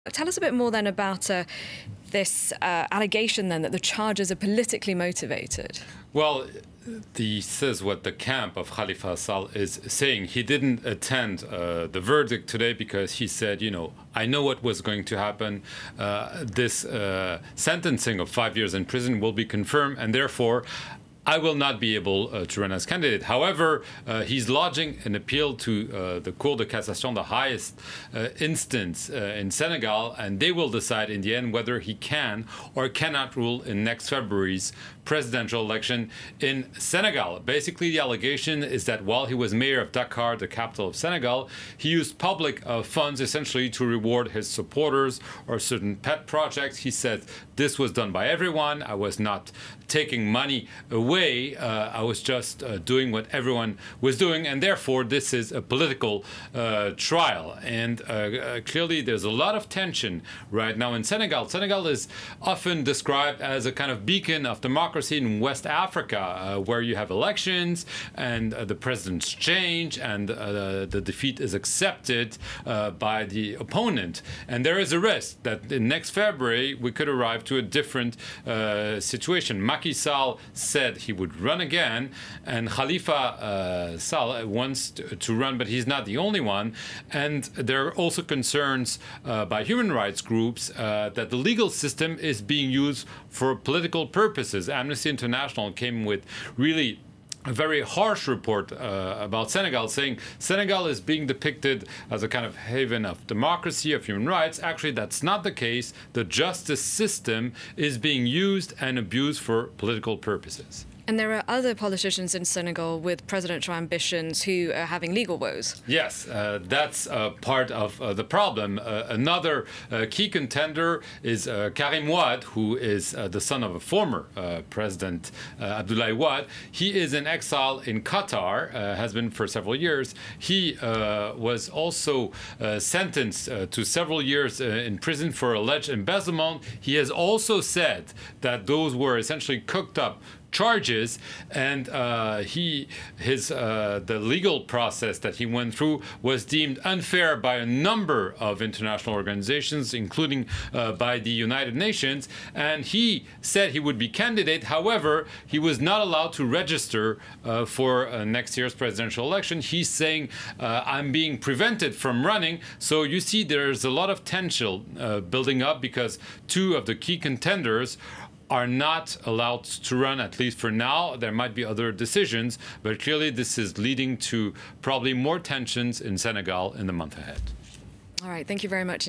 Senegal: Is Khalifa Sall's sentence politically motivated?
In Senegal, a court has upheld Khalifa Sall''s five year prison sentence for fraud. He was arrested last year. He says the charges are politically motivated. Sall can''t run in next year, in which he had been seen as a potential rival to the President.